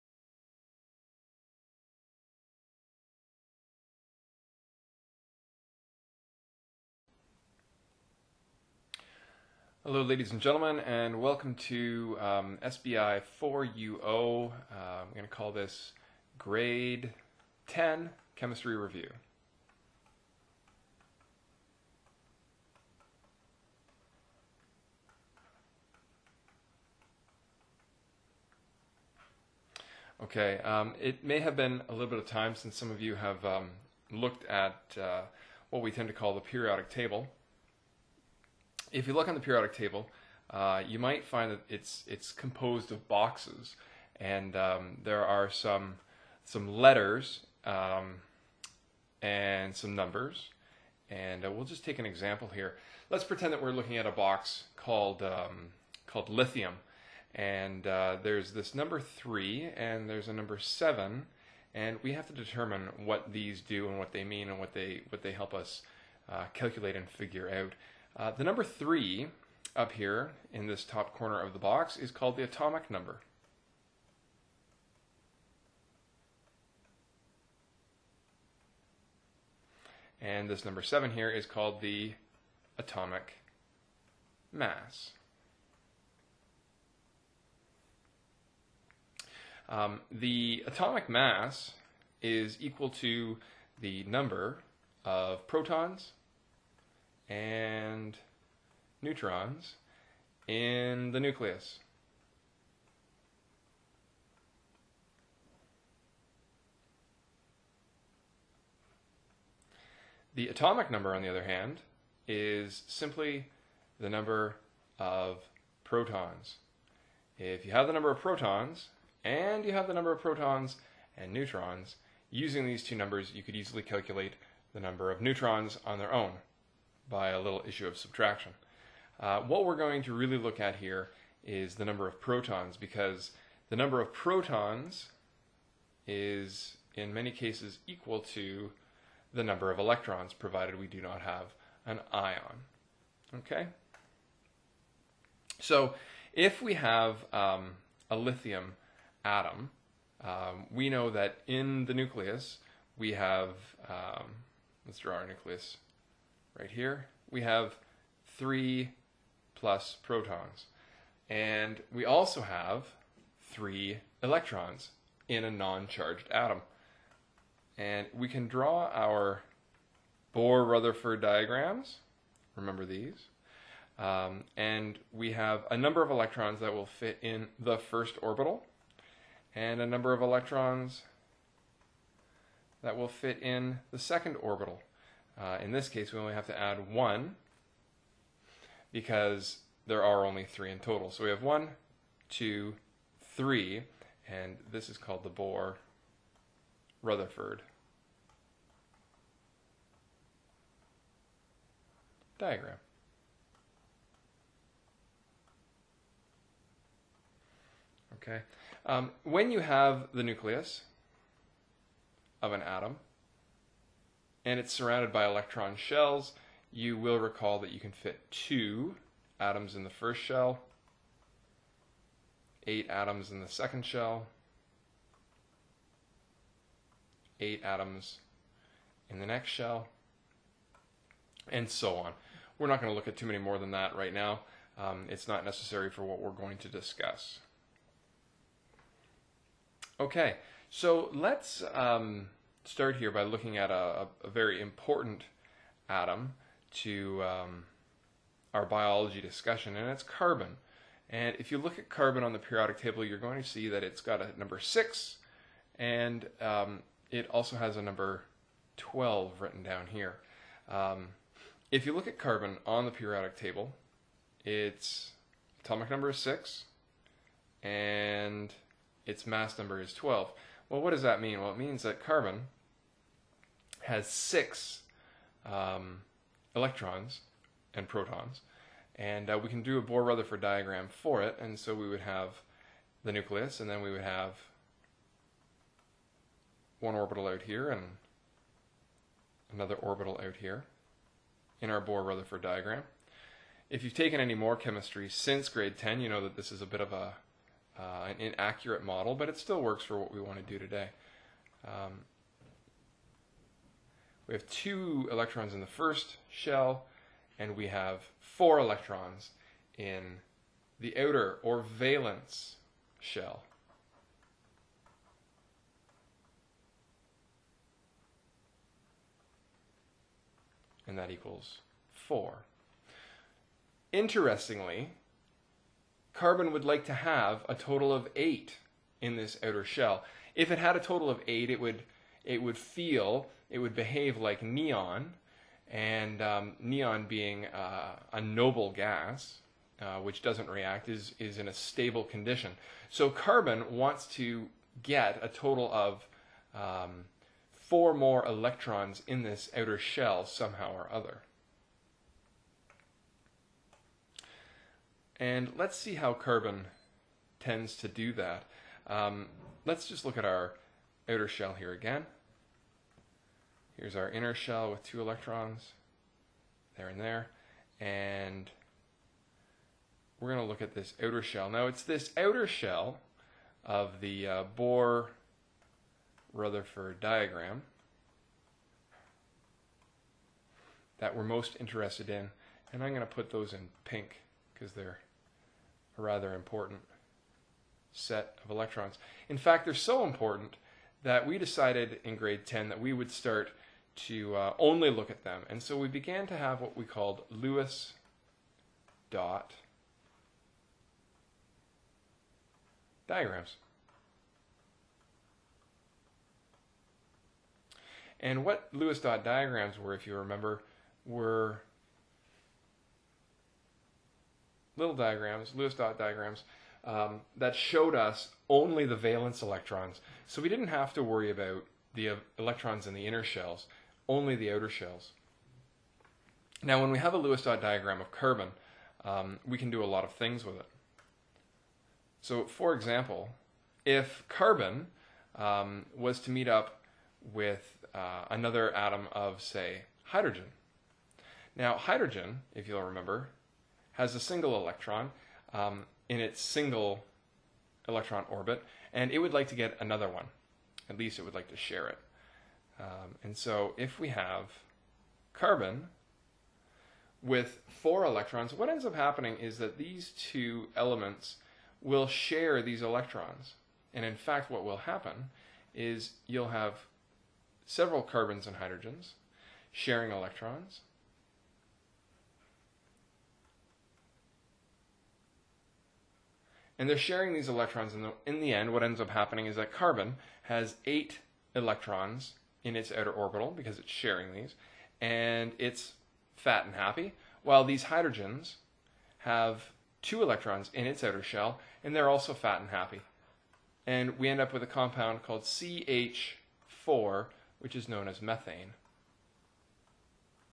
Grade 10 Chemistry Review for SBI-4U
Lesson 1: Quick introductory video for SBI-4U0
Watch on September 5 2012